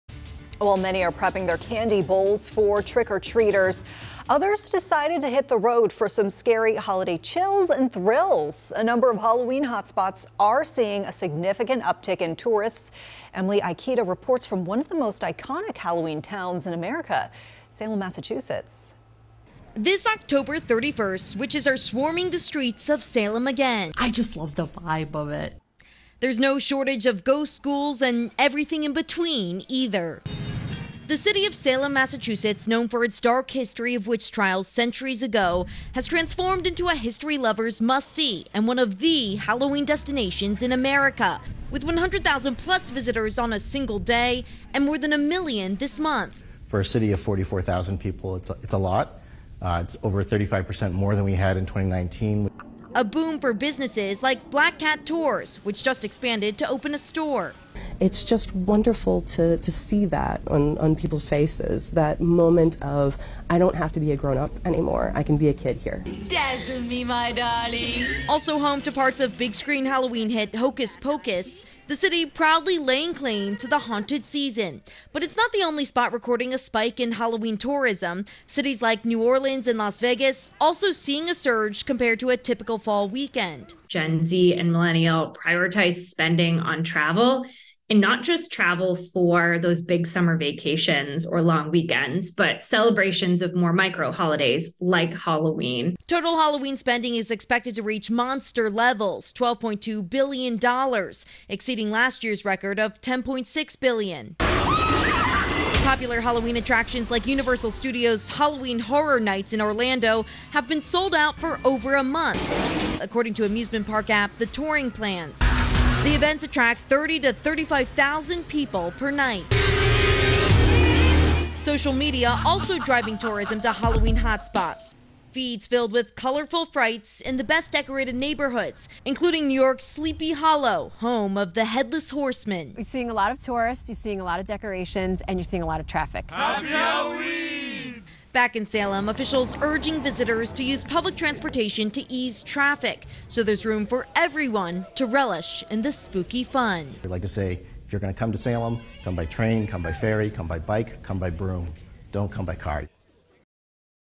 Tourists visiting Halloween haunts more often, research finds
While many Halloween revelers were preparing costumes and bowls of candy for trick-or-treaters, others got their spook on in a different way, hitting the road to hit up haunted hotspots.